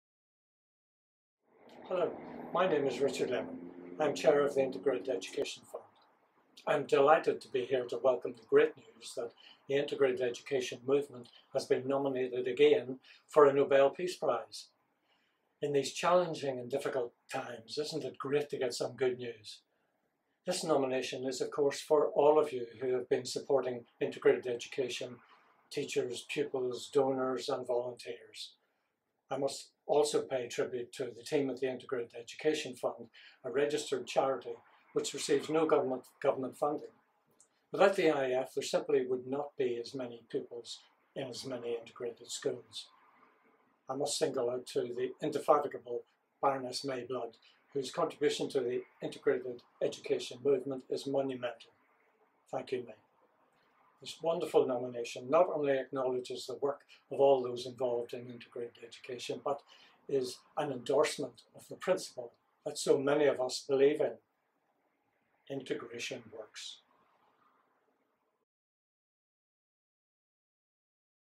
Nobel Peace Prize 2020 Nomination Congratulations from Richard Lemon, Chair of the IEF
Richard Lemon chair of the Integrated Education Fund shares his congratulations to the Integrated Education Movement on their nomination for the 2020 Nobel Peace Prize.